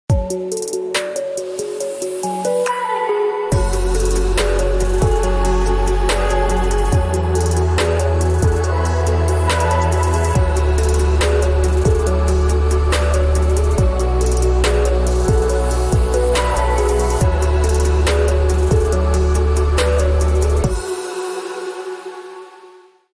Envogue Residence: apartamente perfecte pentru familie, ideale pentru investitie!
Descopera Envogue Residence! Intra pe si gaseste-ti apartamentul ideal! Noi chiar suntem in Militari!

Adrese Birouri Vânzări

Bulevardul Iuliu Maniu 586 – 590, Sector 6. București
 Drumul Ciorogârla 297-299, Sector 6, București

Va așteptăm cu drag!